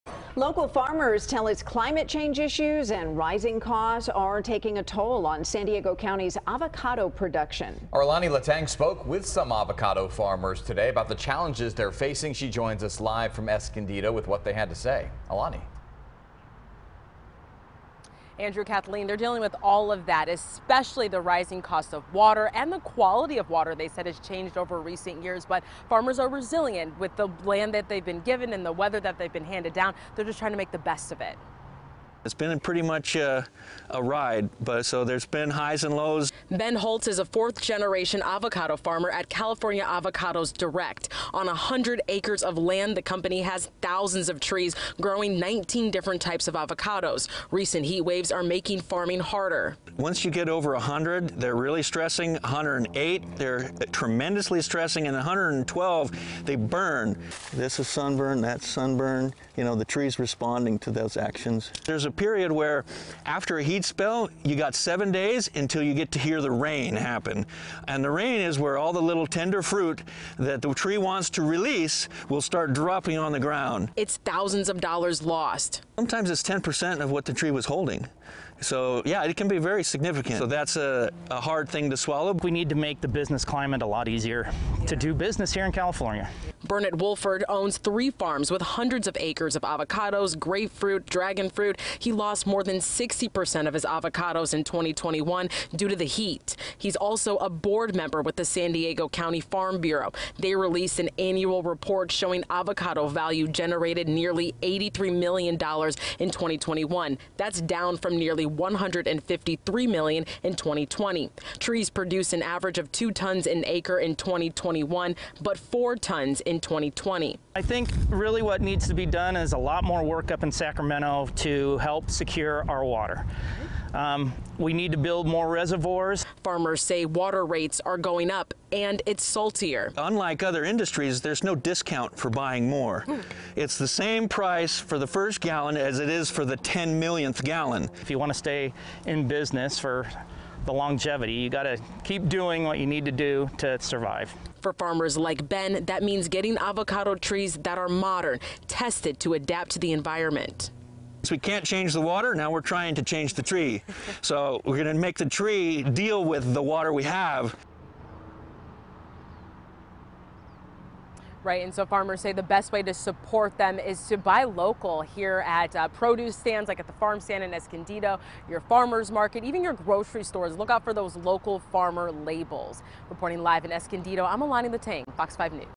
Avocado Growers Facing Challenges
Local farmers tell FOX 5 climate change issues and rising costs are taking a toll on San Diego County's avocado production.